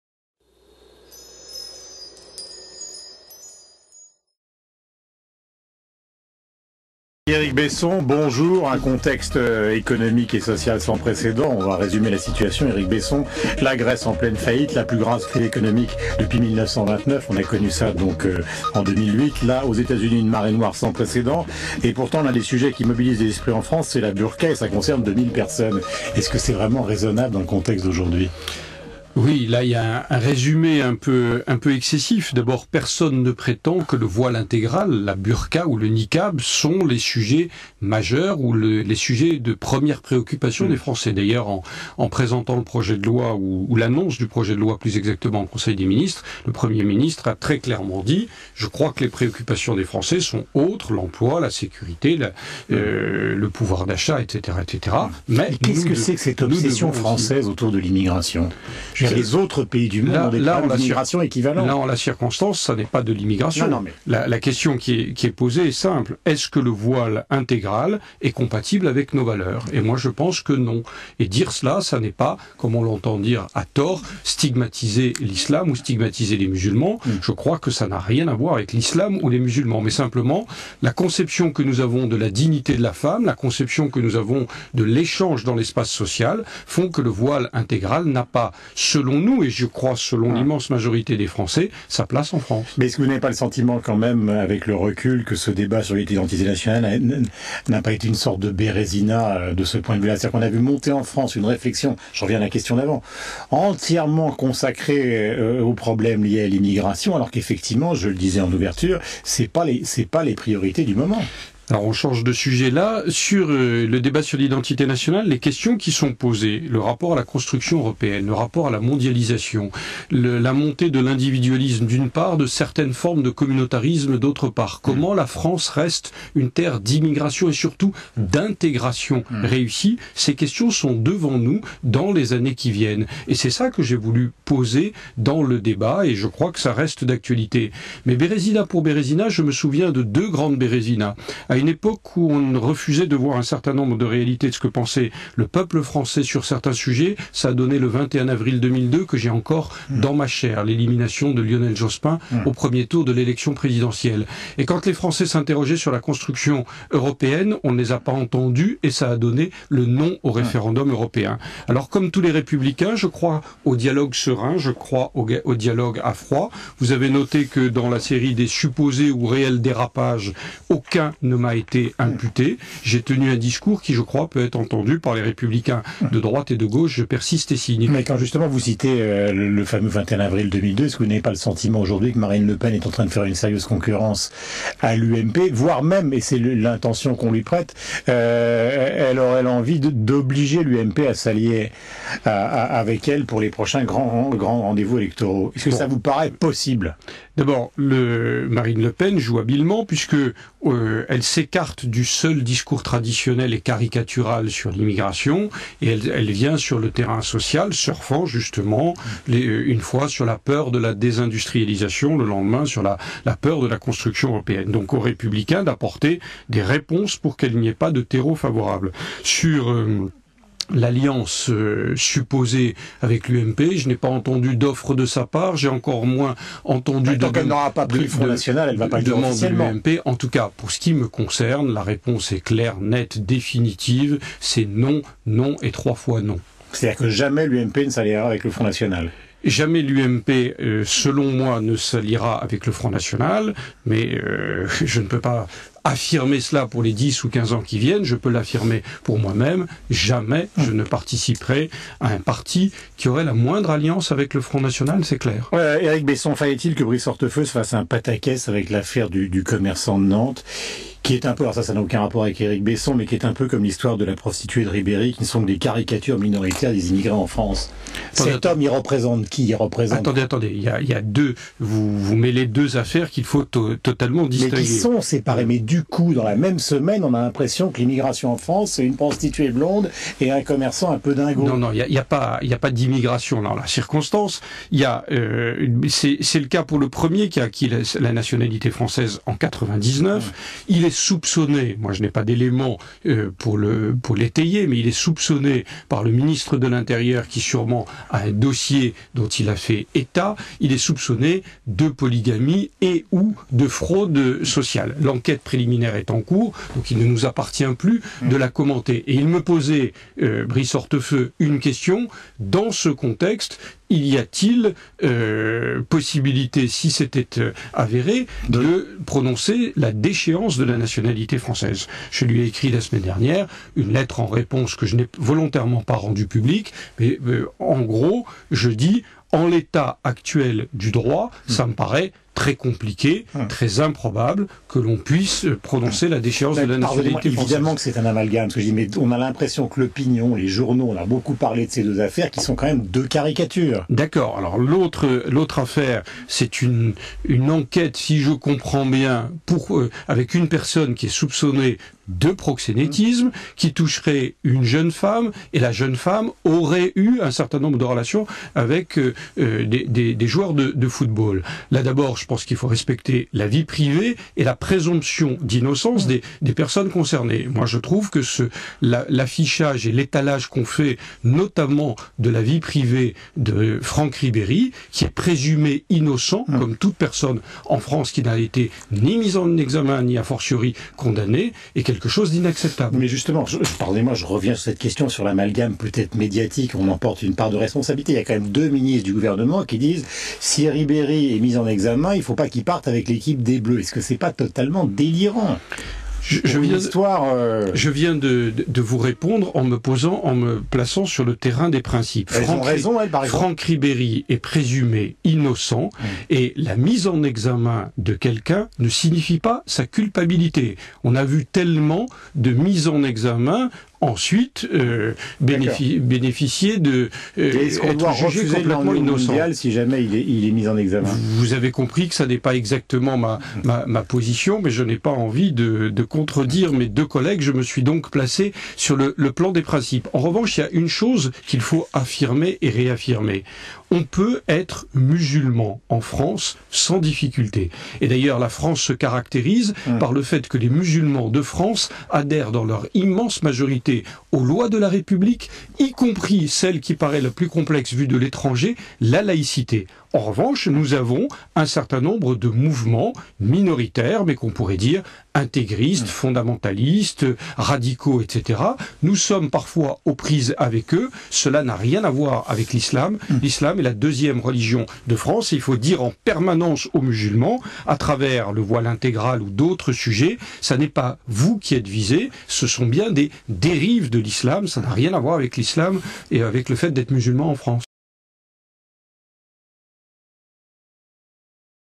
Eric Besson, invité de Guillaume Durand
Eric Besson, ministre de l'Immigration, de l'Intégration, de l'Identité nationale et du Développement solidaire, était l'invité de Guillaume Durand, à 8h30 sur Radio Classique.

Retrouvez toutes les vidéos en intégralité sur radioclassique.tv